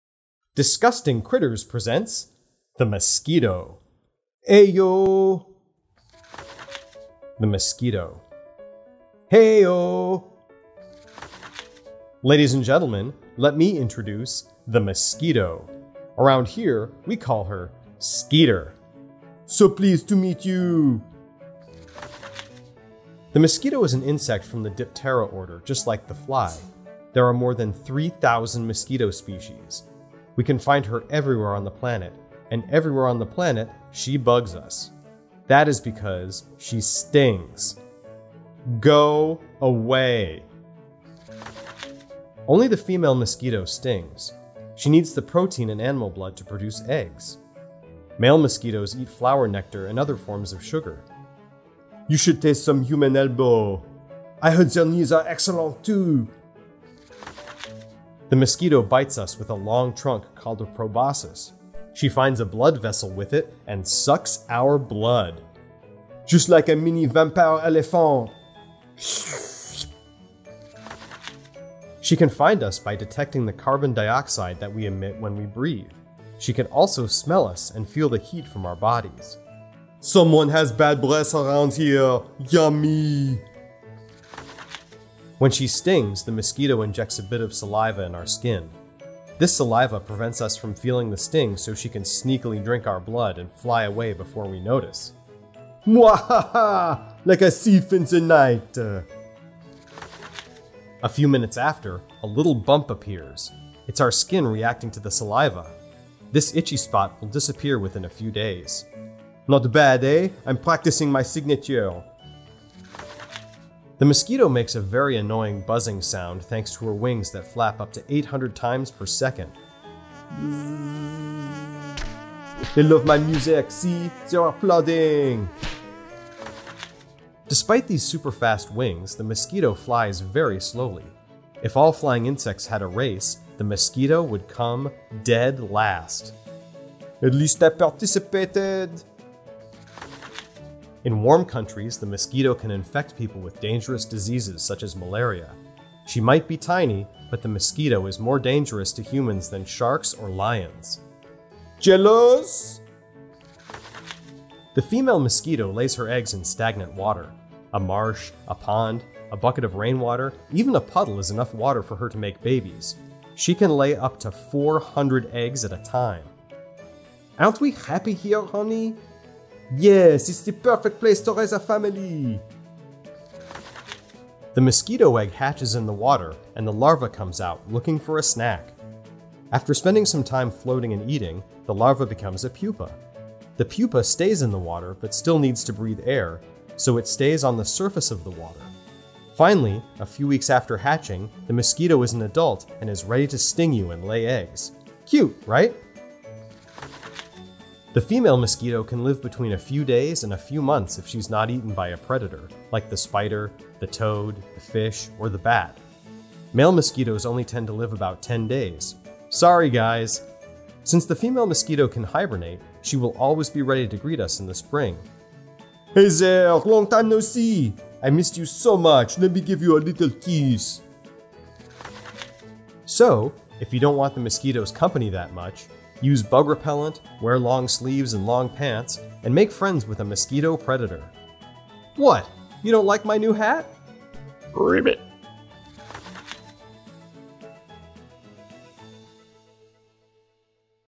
Disgusting Critters presents The Mosquito - Book Read Aloud
Hilarious illustrated nonfiction about mosquitos perfect for beginning readers. Disgusting Critters presents The Mosquito by Elise Gravel with conversational text and silly illustrations will have you up all night reading about the most annoying bug on Earth!

Fast mosquito facts:
  •  Distinctive trait: Leaving annoying itchy bites
  •  Diet: Your blood (and nectar and plant juice)
  •  Special talent: Making a terrible whining sound in your ear

The Mosquito covers habitat (mosquitos live everywhere except Antarctica and Iceland!), species (over 3,500!), history (the oldest recorded mosquito was 79 million years ago!) and much more. Although silly and off-the-wall, The Mosquito contains factual information that will both amuse and teach at the same time.

Kids learn about mosquitos in this video with music, sound effects, and silly voices.

It's a great book and you should pick up a copy